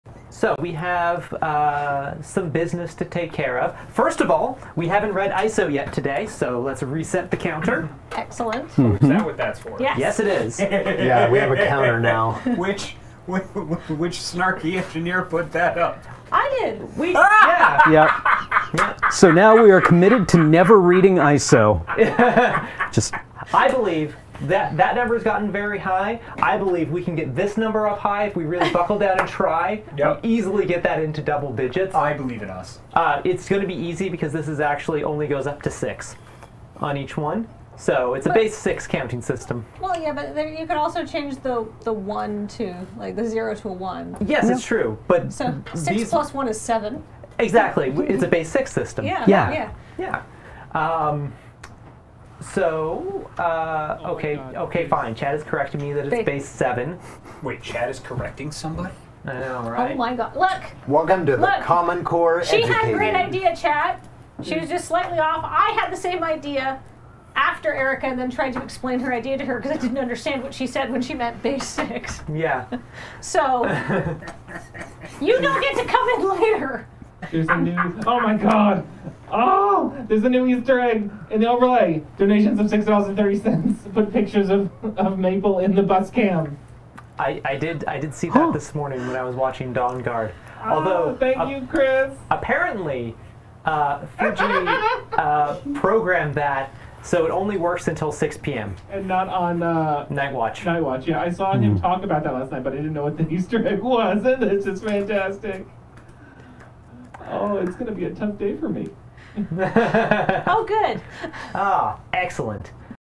DB2022 - Erika resets the Reading ISO counter
Erika resets the ISO counter. And Joe finds out about the Maple Easter Egg.